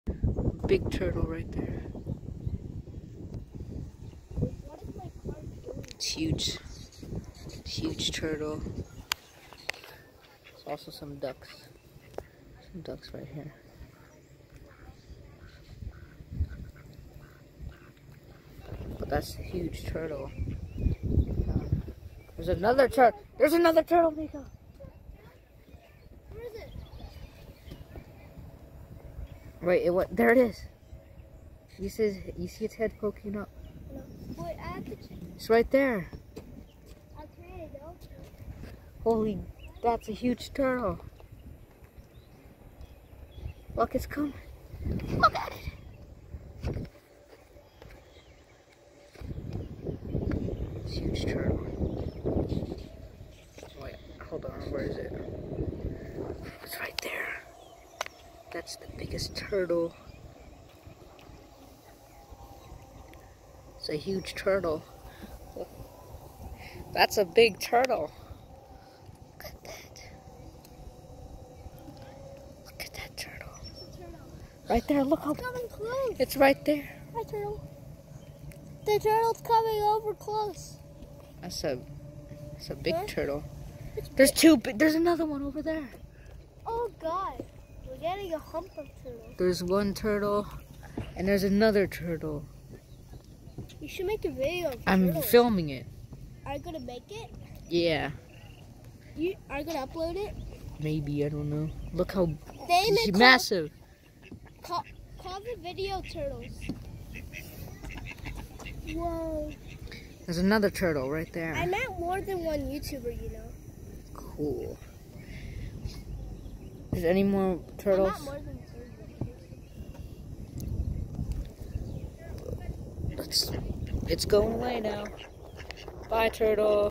Turtles (Happy Easter!)
Saw some turtles while egg hunting